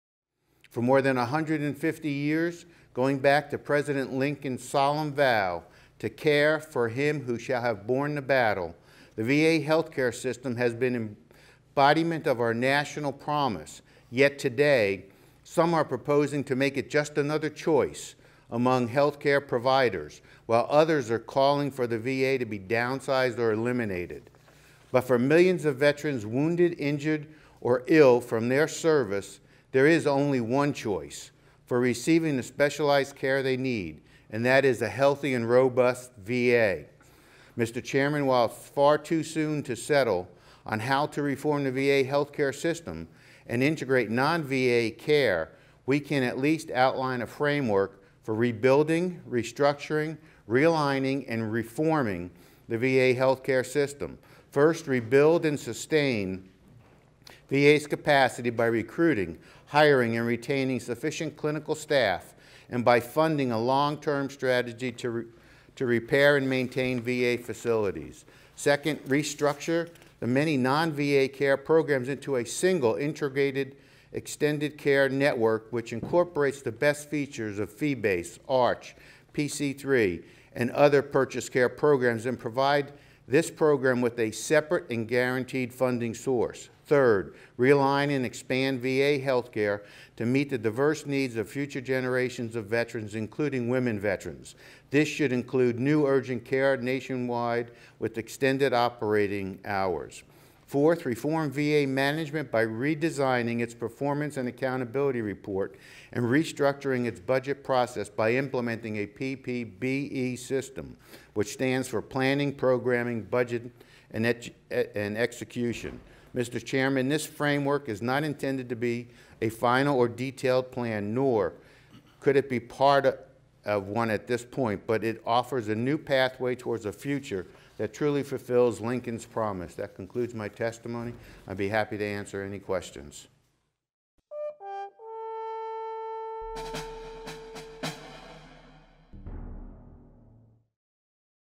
DAV lays out framework for reforming VA health care system
DAV National Legislative Director Joe Violante delivers testimony before the Senate Veterans' Affairs Committee concerning the VA Choice Card program May 12.